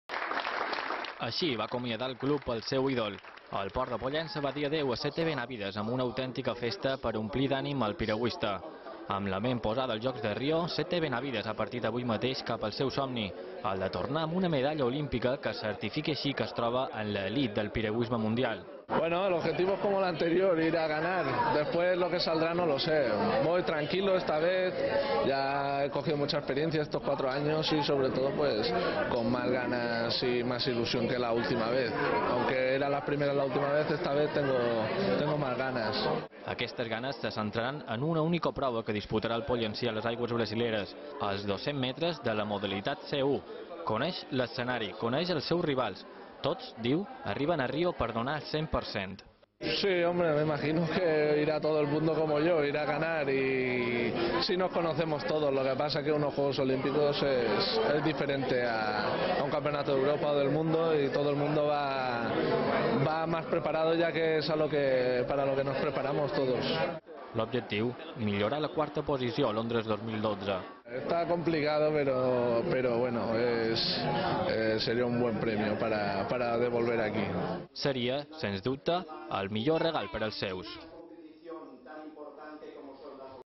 Pollença dóna ànims a Sete Benavides en la lluita per a una medalla olímpica
El port de Pollença va dir adéu a Sete Benavides amb una autèntica festa per omplir d'ànim el piragüista. Amb la ment posada als Jocs de Rio, Sete Benavides ha partit avui mateix cap al seu somni: tornar amb una medalla olímpica que certifiqui així que es troba en la elit del piragüisme mundial.